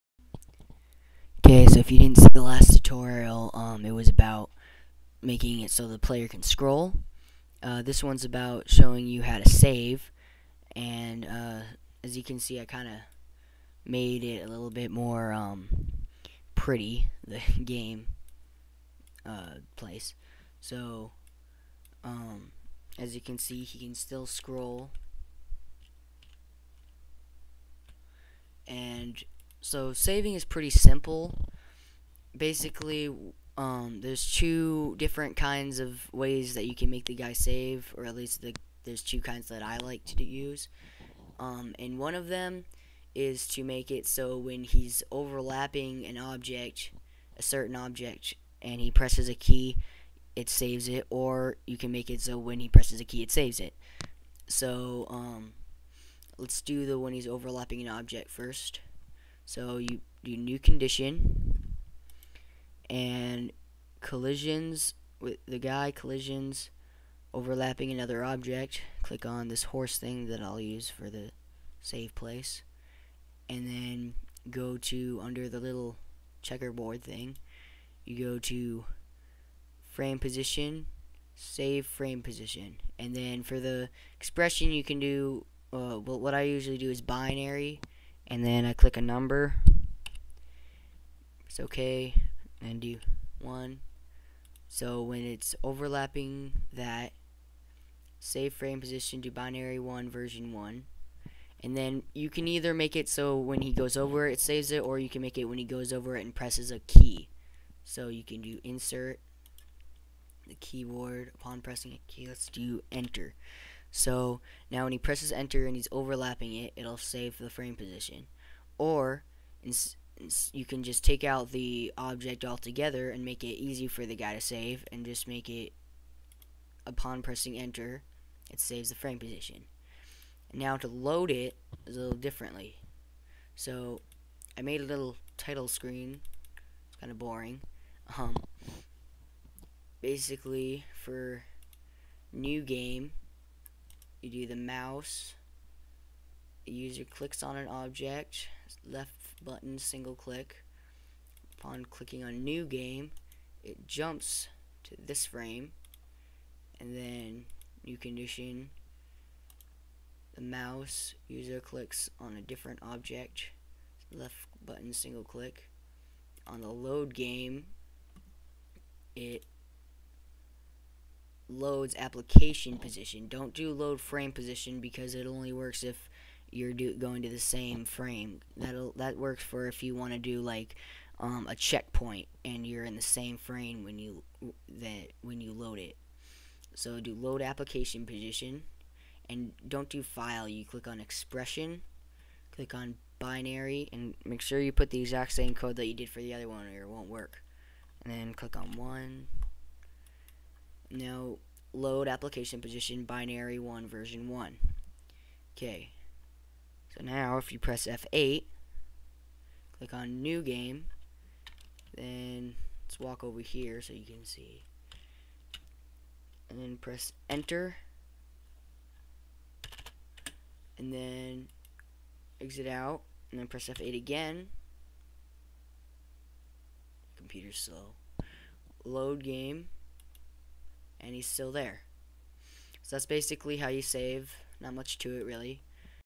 MMF2 Tutorial: Making an RPG game part 2: Saving
This is part 2 of my RPG tutorial series. This tutorial is for beginners and shows you two simple ways you can make the player be able to save. Of course, saving isn't just for RPG games. As you'll find out when you watch the video I am actually talking this time, having to put annotations on every vid was getting annoying and I got a better mic now.